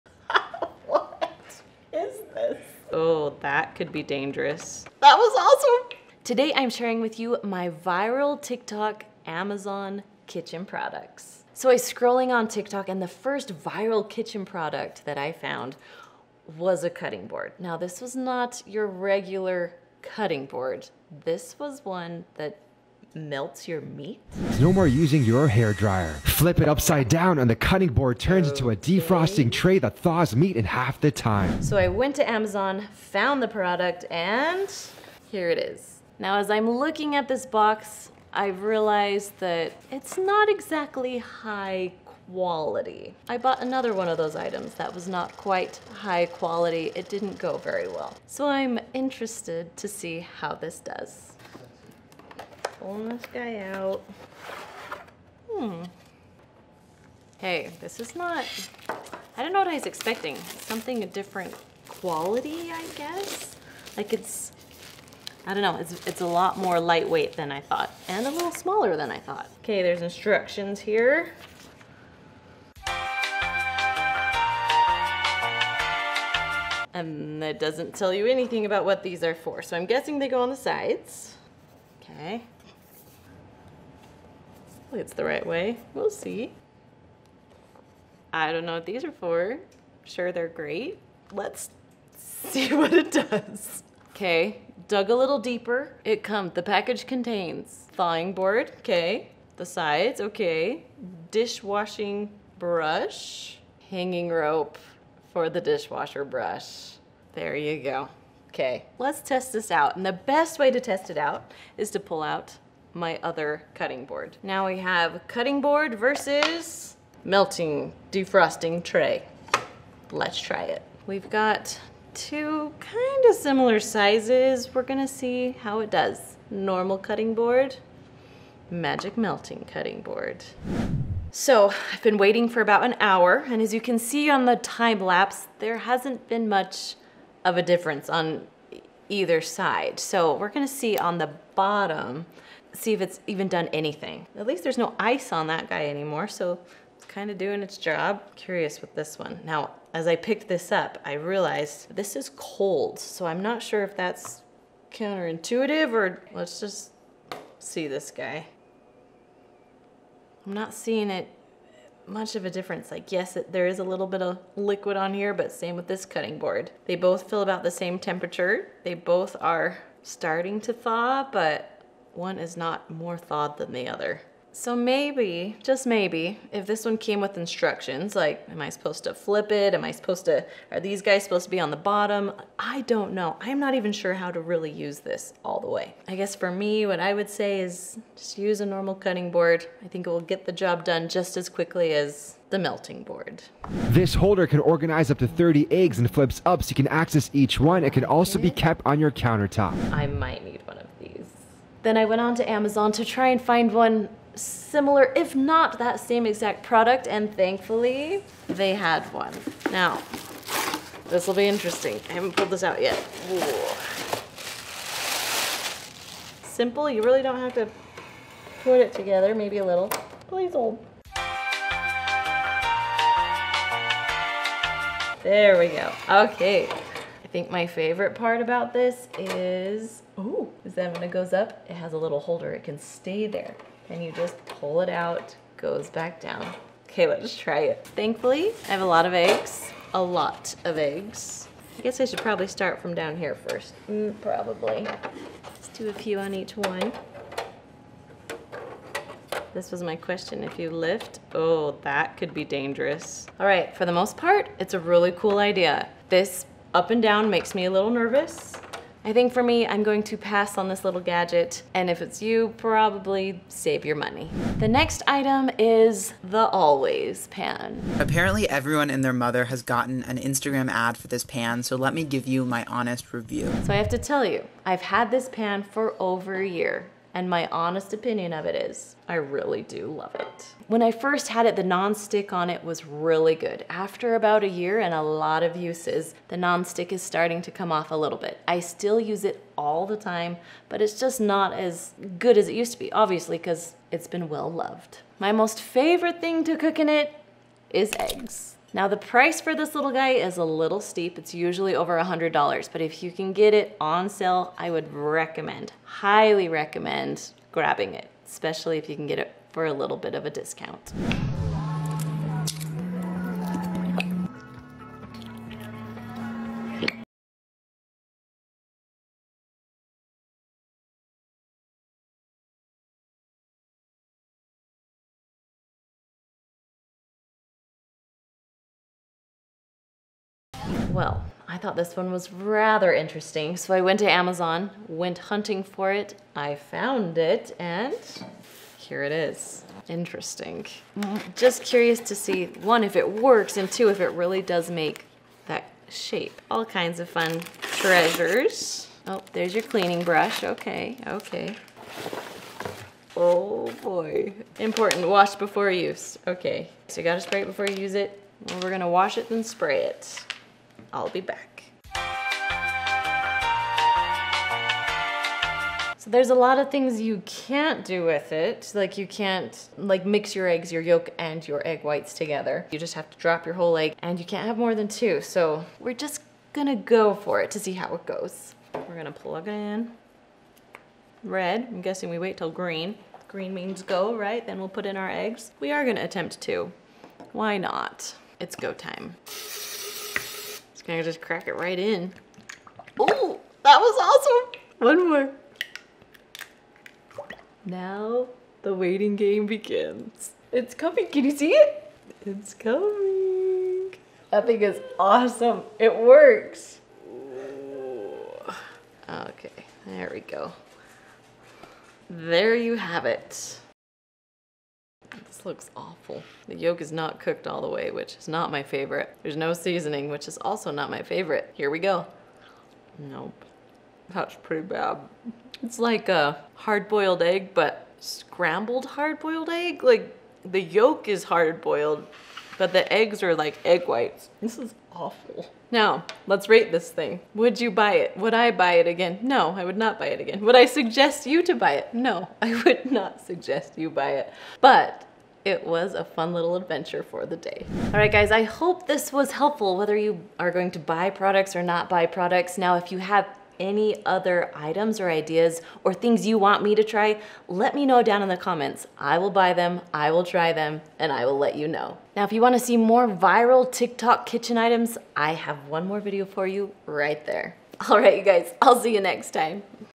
I Bought Viral Kitchen Products from AMAZON! - Do They Even Work?
Don't tell me I'm the only one watching Tik Tok and obsessed with trying out all the viral kitchen items.  If you don't want to buy them all, don't worry I got you covered!  Watch this video and see if these are worth all the hype!

00:00 Viral Tik Tok Kitchen Items

00:12 Melting Cutting Board

03:12 Egg Container

04:16 Our Place Pan

05:11 Rollie Egg Cooker

Want more TIK TOK Kitchen Items?  Find them here:
I Bought Viral Tik Tok Kitchen Items